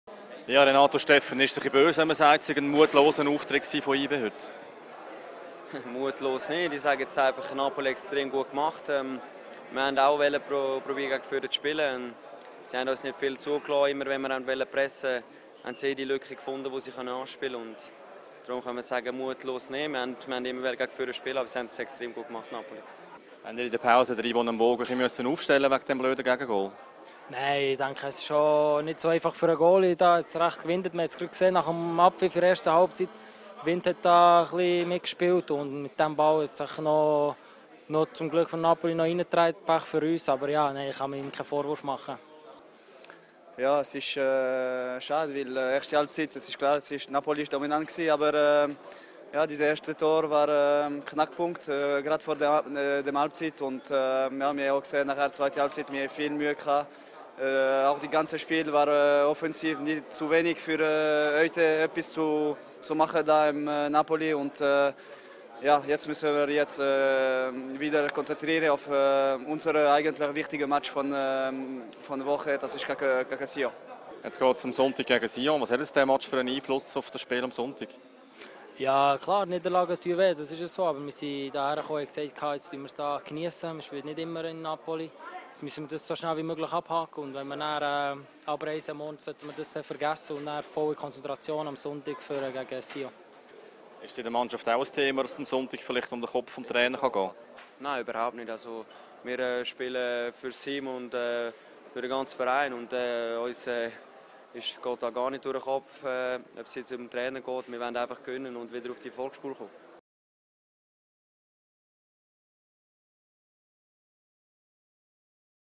YB-Spieler nach Napoli-Pleite
Das sagen die YB-Spieler nach der 0:3 Niederlage gegen Napoli.

Sport